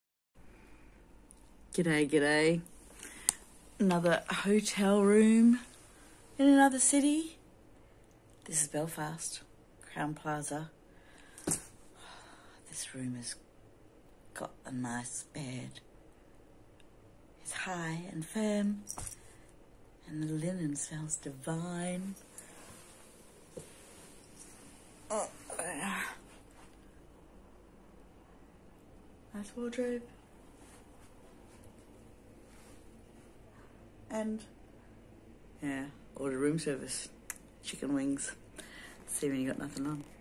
Aussie in Ireland : Hotel Room Belfast Edition
Another Irish city, another hotel room. Gorgeous food!